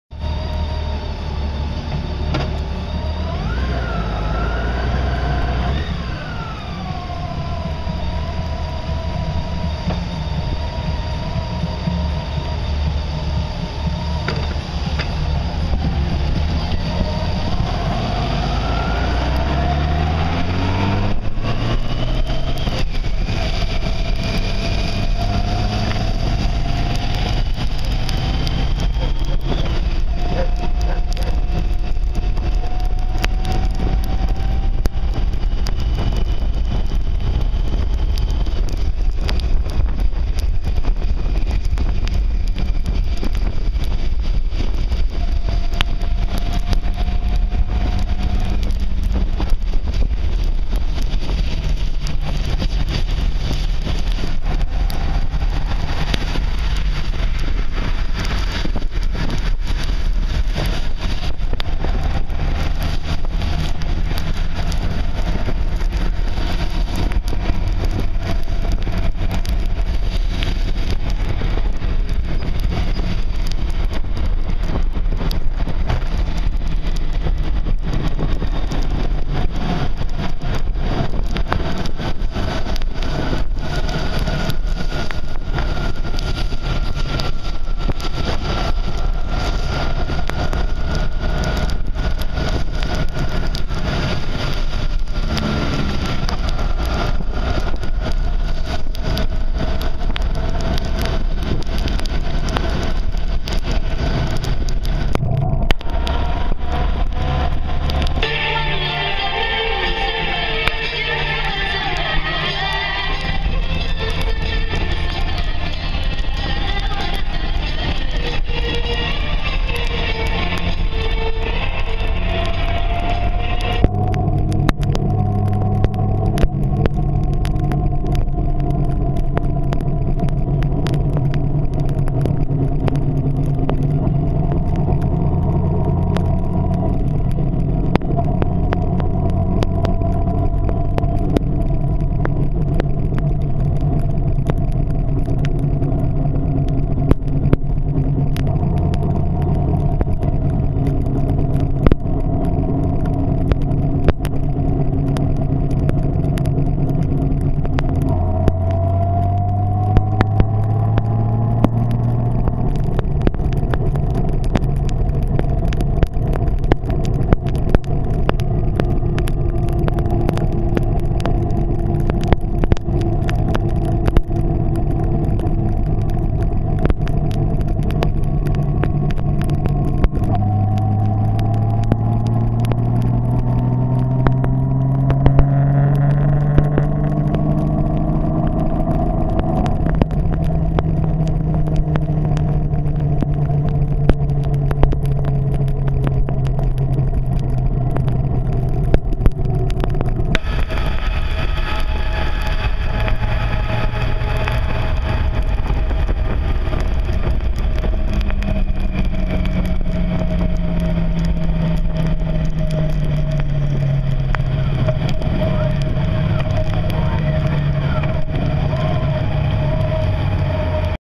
Goldwing to Antalya PART 3
3 Saudis and 4 Manavgat team wingres ride from Egirdir to  Antalya.